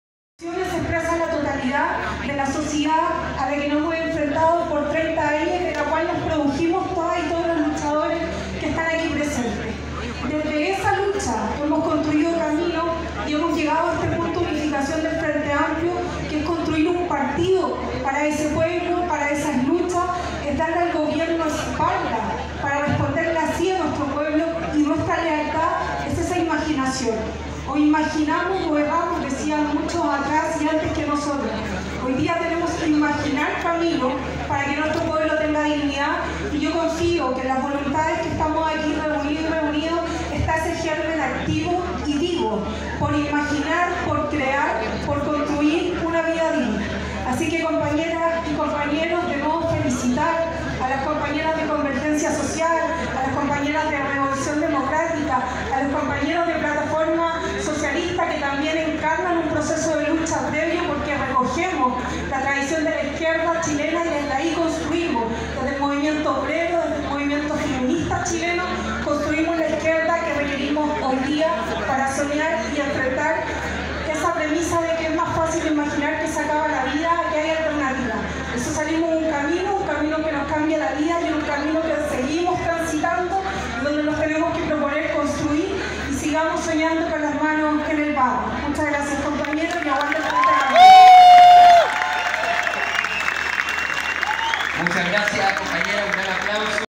Plebiscito del Frente Amplio para fusionar sus partidos, en uno solo!
Apruebo: 80%
Rechazo: 19%

Pronto Convergencia Social, Revolucion Democratica, Plataforma Socialista y Comunes, seran un solo partido de apoyo al Presidente Gabriel Boric en Chile.